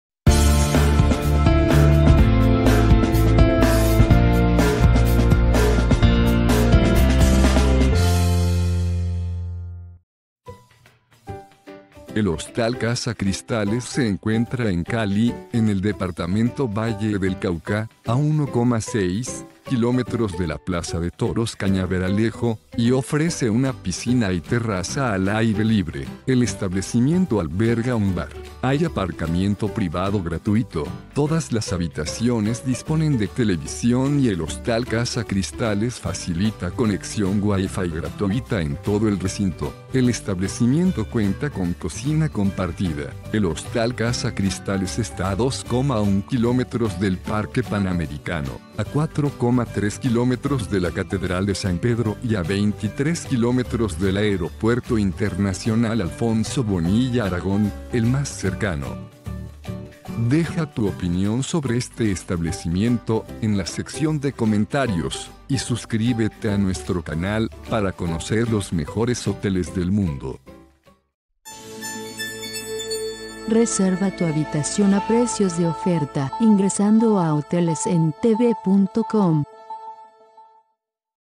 Hostal Casa Cristales en Cali
Hostal Casa Cristales es un Albergue de 8 habitaciones localizado en calle 8 oeste 35-24.